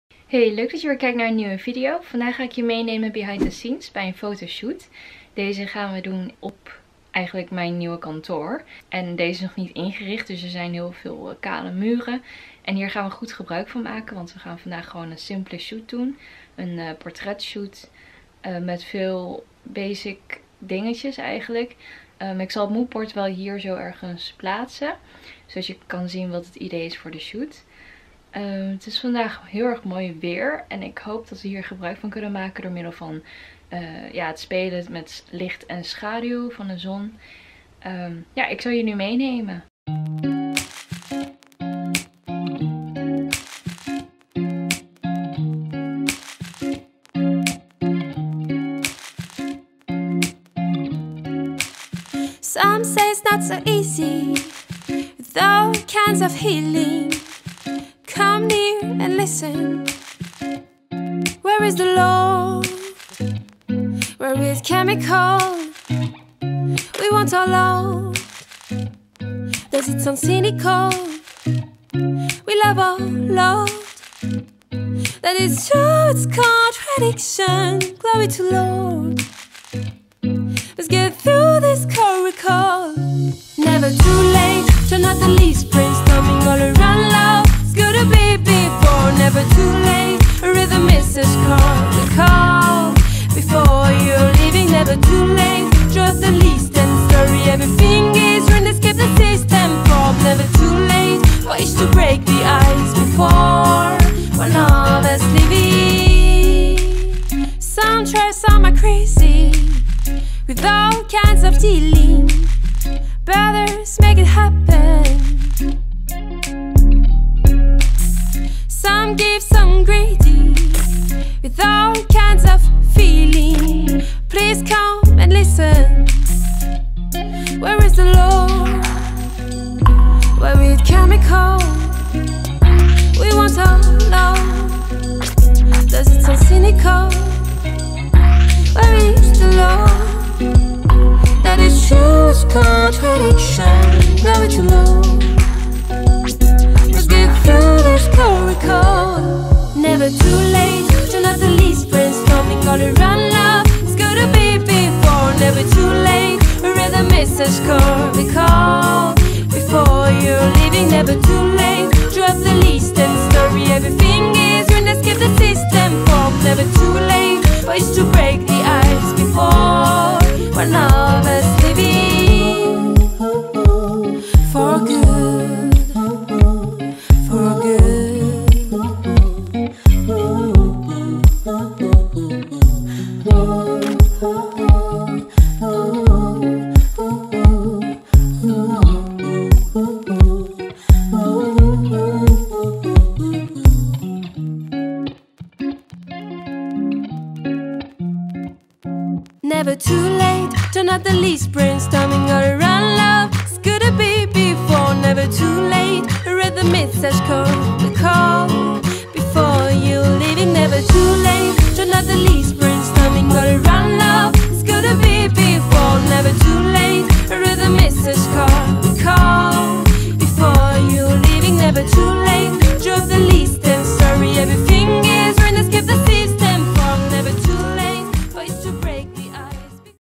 Binnen fotoshoot met natuurlijk licht - Behind the scenes
In deze video neem ik je mee behind the scenes bij een fotoshoot binnen met natuurlijk licht. Leuk om eens voor de verandering wat meer basic dingetjes te schieten!

Model: Evri (NJ Model Management)
MUAH: Xiu Yun Yu